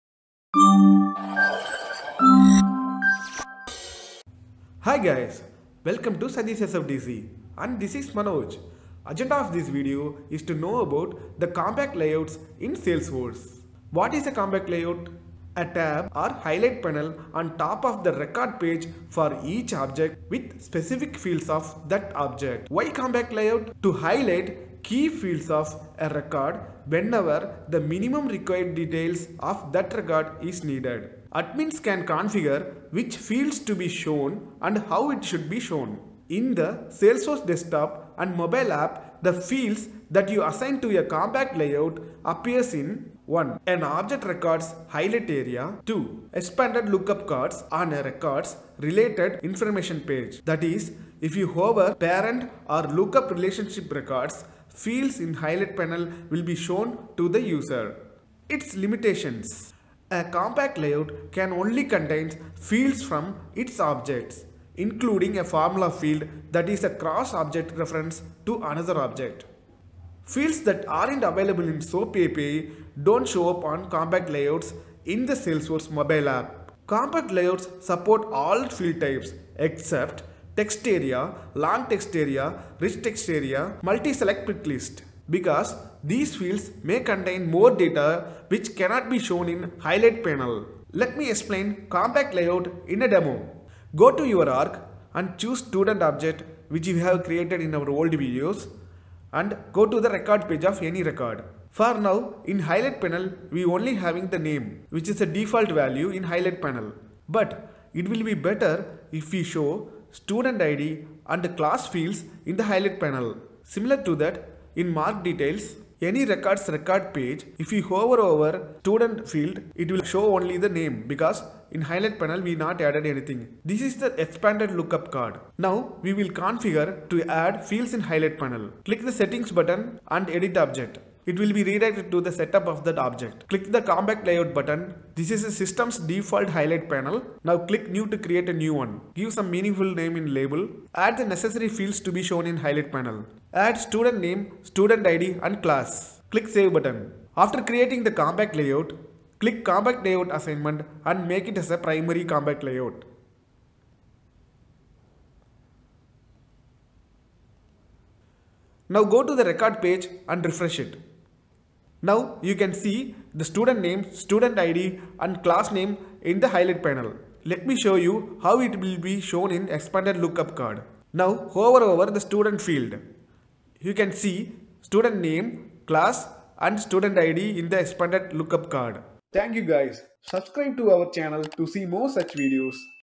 What is Compact Layouts in Salesforce And How to Create Them?
What is a compact layout? How do they benefit our users? Are they all bad? In this Salesforce Compact Layouts tutorial, I go over all things related to creating and using compact layouts in your org.

You will learn what is Compact Layouts in Salesforce and How to Create blog posts. I will show you how to create layouts in a compact setup, what are the benefits of compact layout configuration and why a compact setup could be a good idea for you. Salesforce Compact Layouts create a better user experience and make navigating the application easier and faster.